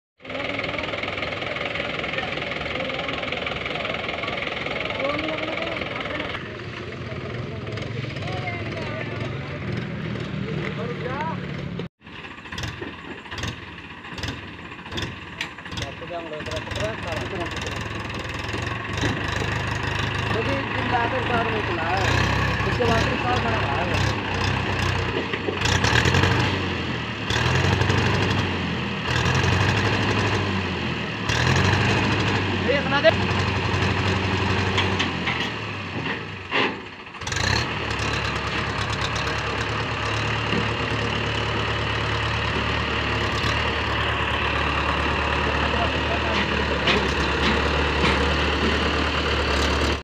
Mahindra 585 XP Plus, Mahindra 575 and powertrac Tractor fail to rescue Eicher truck
Mahindra 585, Mahindra 575 and powertrac Tractor fail to rescue truck. ‎@Mohd. Shahban